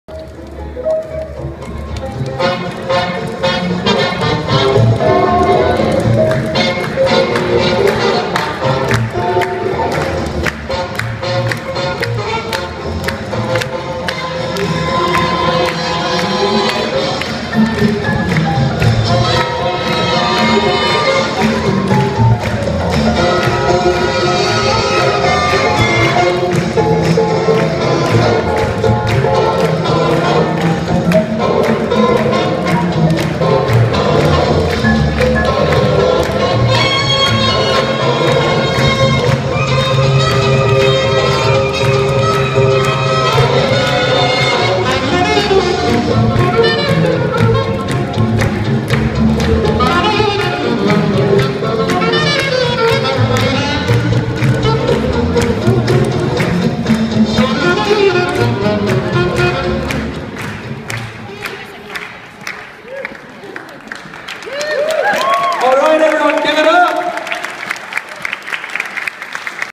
Jack & Jill All-Skate 1 - Chicago Green Light Exchange
Our six couples dance together before they compete.